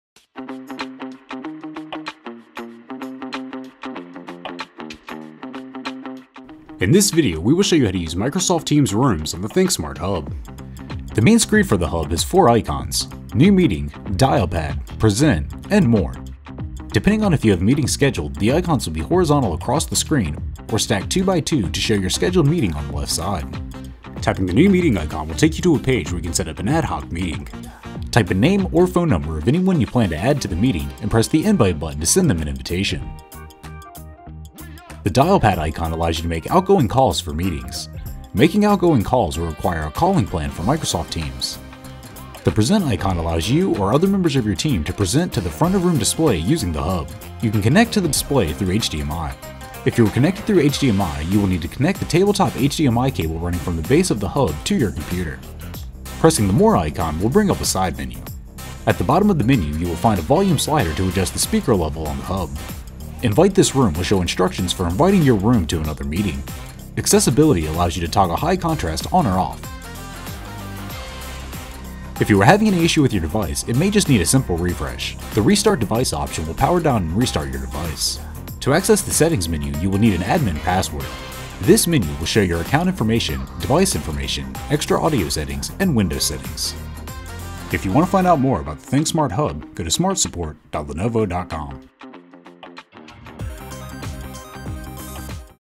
ThinkSmart Hub - How to Use Microsoft Teams
This video will demonstrate how to use the ThinkSmart Hub when it is configured for  Microsoft Teams Rooms.